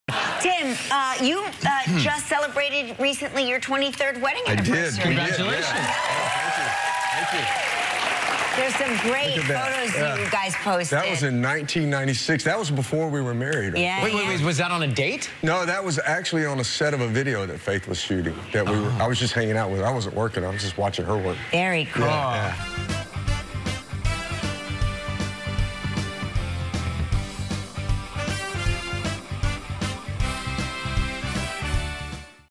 Tim McGraw and Faith Hill's 23rd Anniversary
Tim McGraw talks about his 23rd anniversary with Faith Hill.